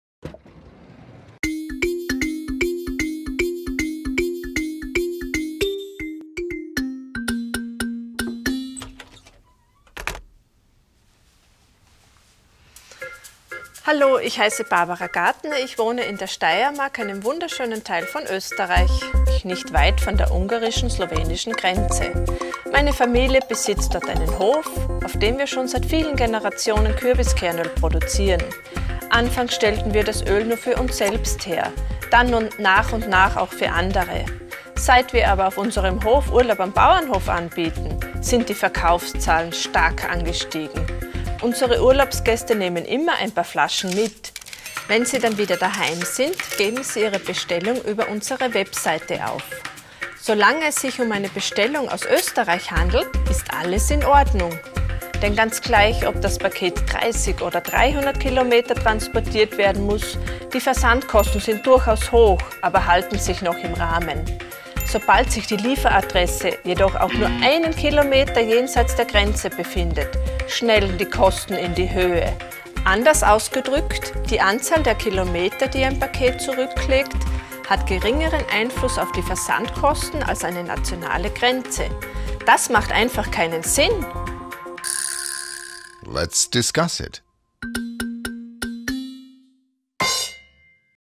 Tell us your story - Citizens tell their stories - Barbara Gartner (Austria)
Barbara Gartner is one of the winners of the EU-wide competition 'Tell us your story' calling on citizens and businesses to share their experiences of the Single Market. More than 150 entries were received and 5 stories, including Barbara's, were selected. The five winners are now invited to present their stories at the Single Market Forum in Krakow (Poland) from 2 to 4 October 2011. The Forum will examine the state of the Single European Market, the transposition and application of EU laws and exchange best practices on solving problems with cross-border exchanges in the EU. The deadline for registration is 25 September